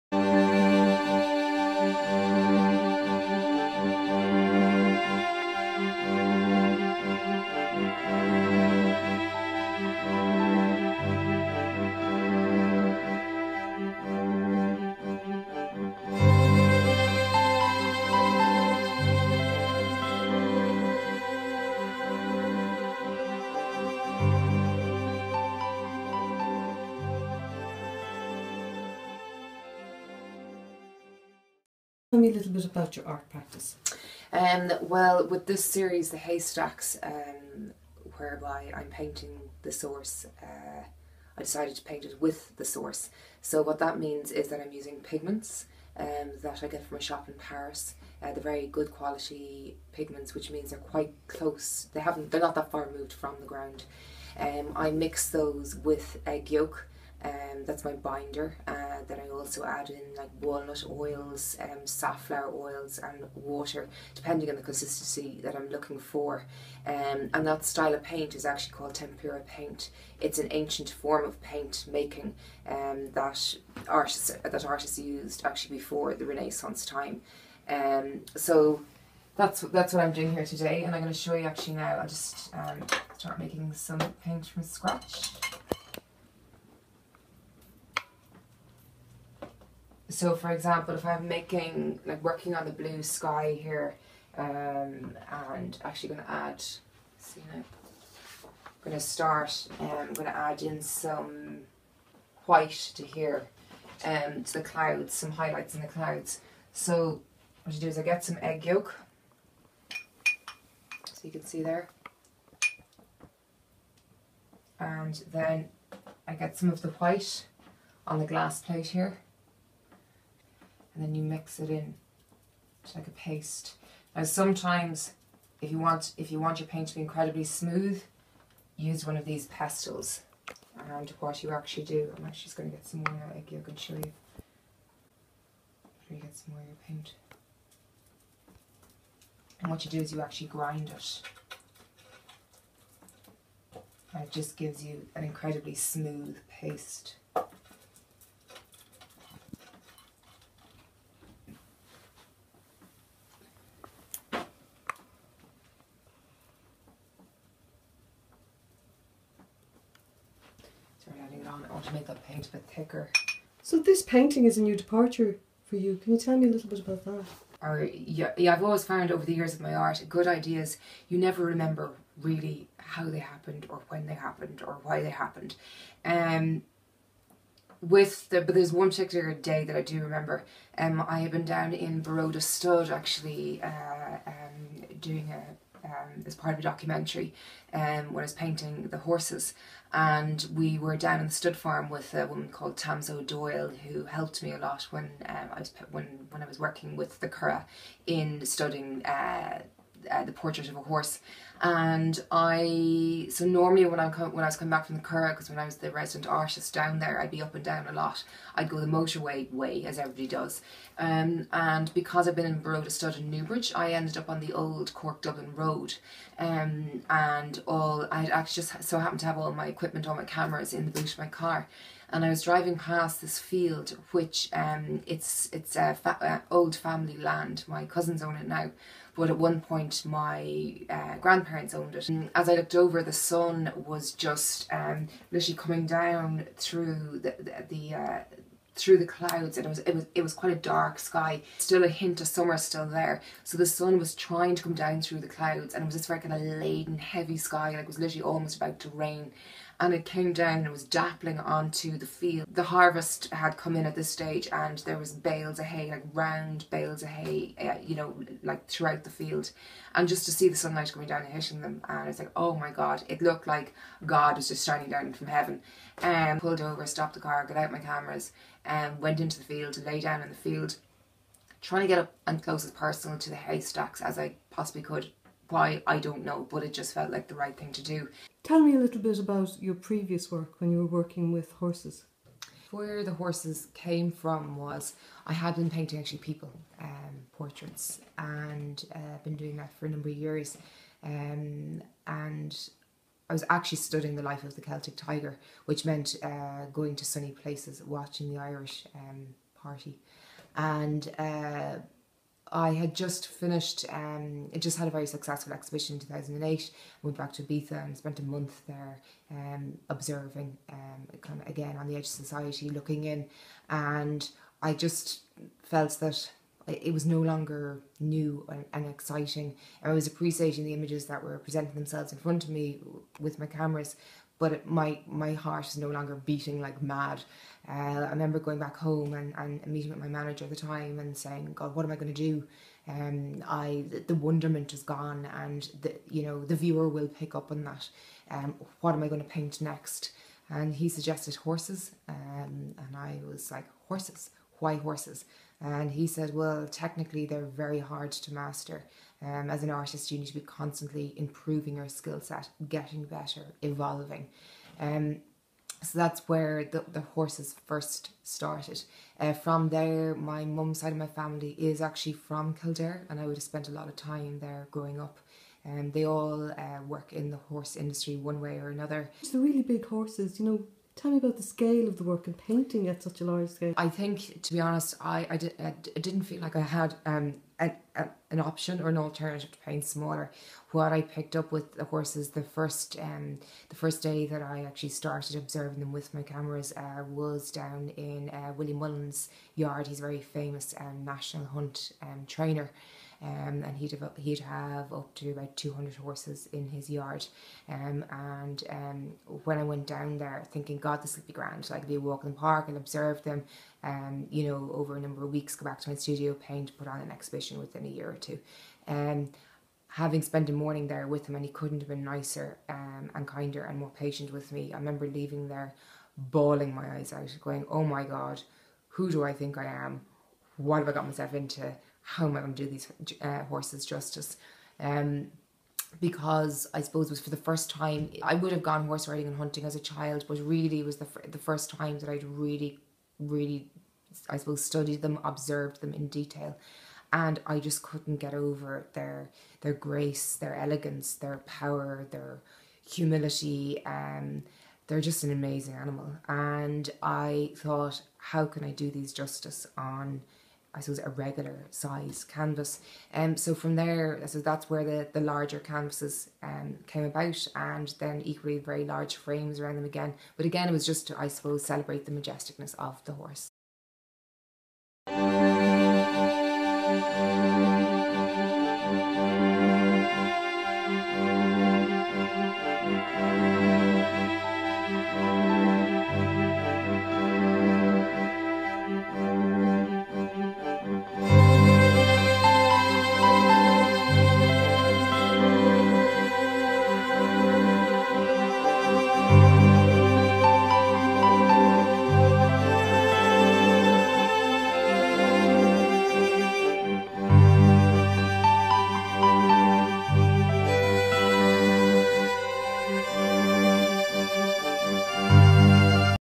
Visual Artist Chara Nagle in her Studio
Maebh O'Regan in conversation with Chara Nagle about projects past and present